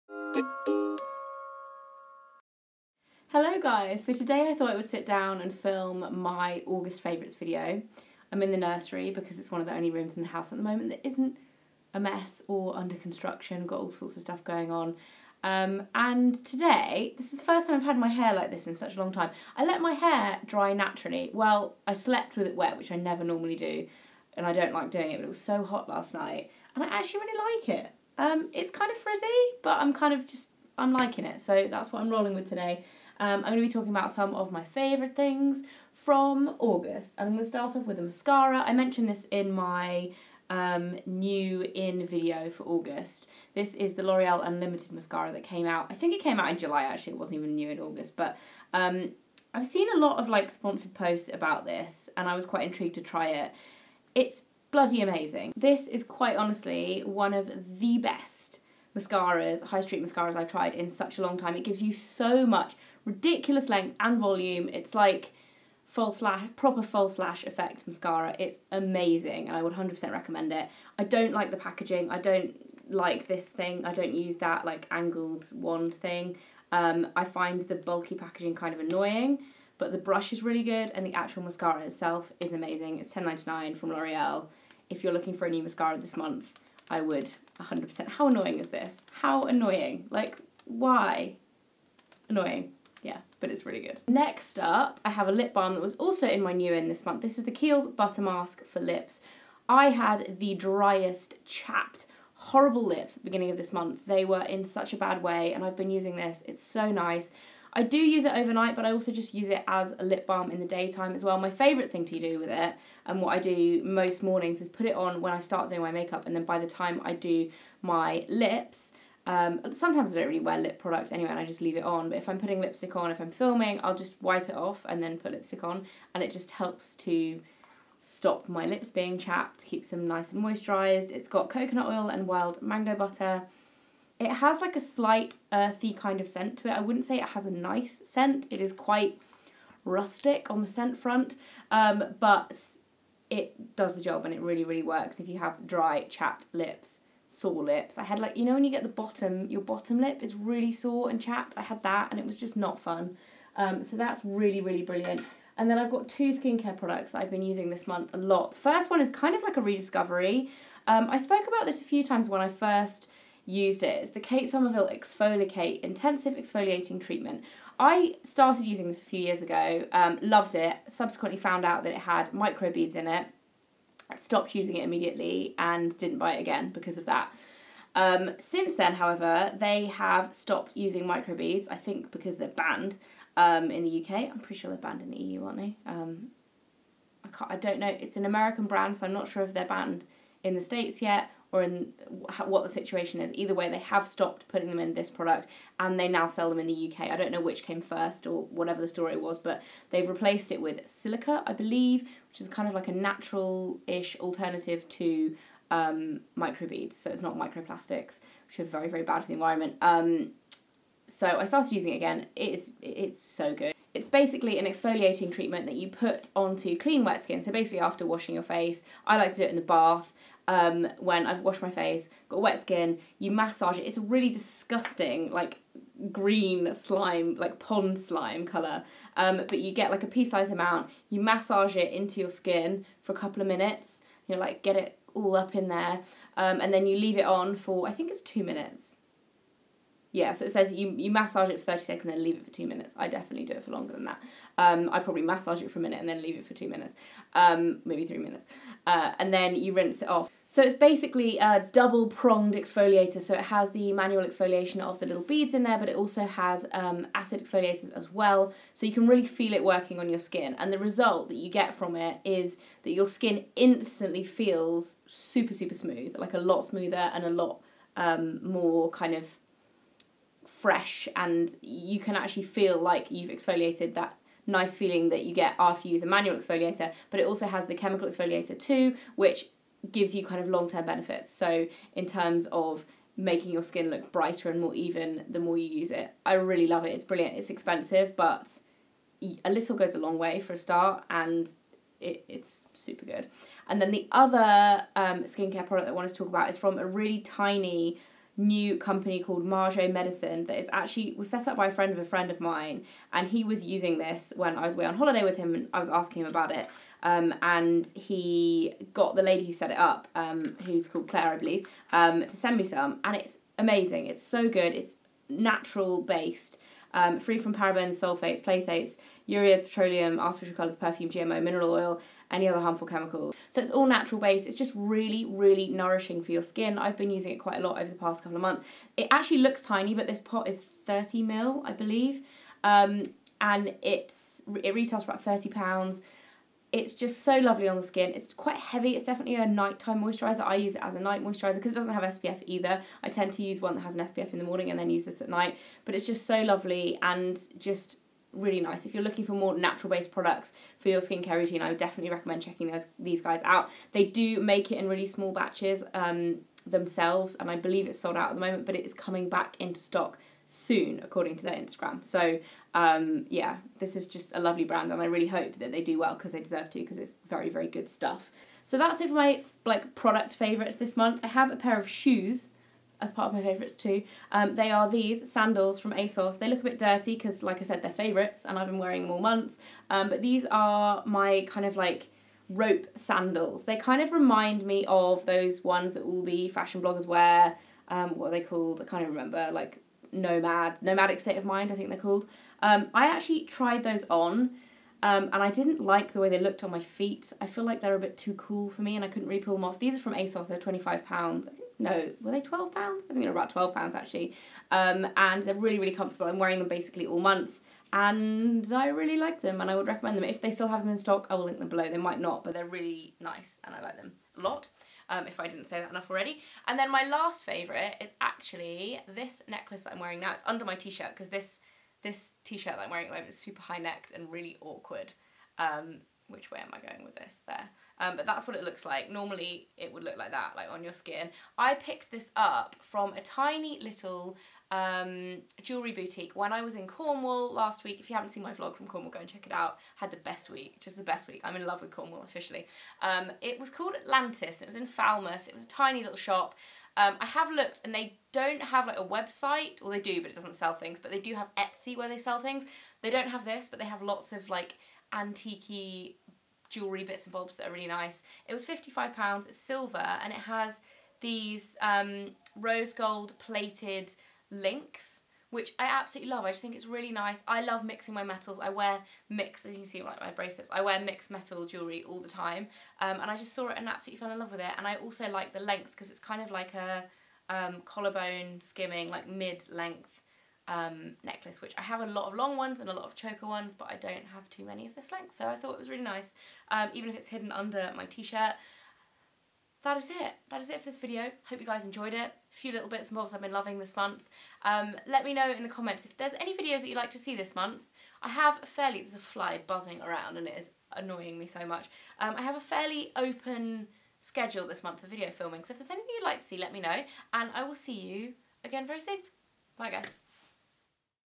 August FAVOURITES! | Fleur De Force
Faves time again! (Where has August gone?! 🙈) These are my favourite beauty and fashion bits this month, what are yours?

PRODUCTS FEATURED:
L’Oreal Unlimited Mascara